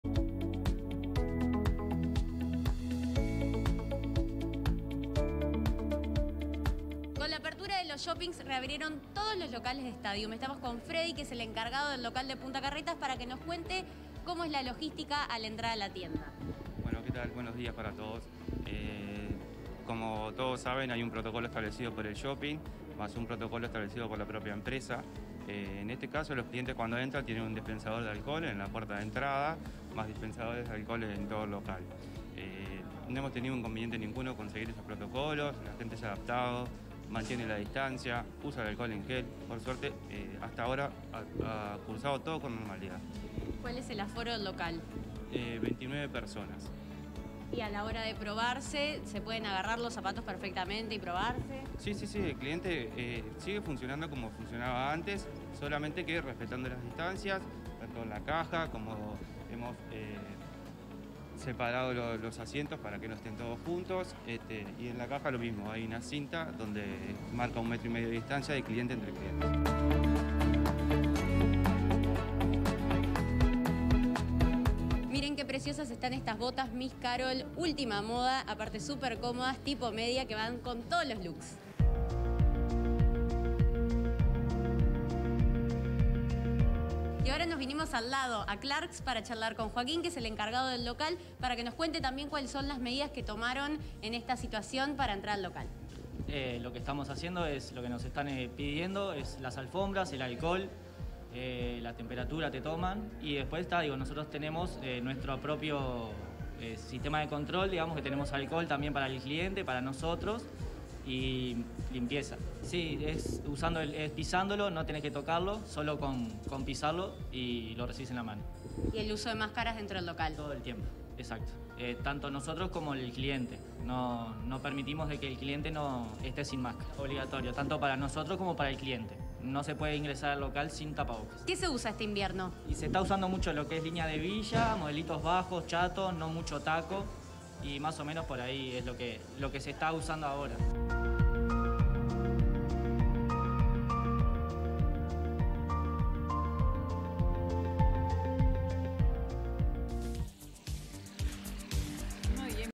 Reabrieron los centros comerciales con sus protocolos sanitarios
Cami dio una vuelta por los shoppings para conocer cómo se ajustaron a al nueva normalidad.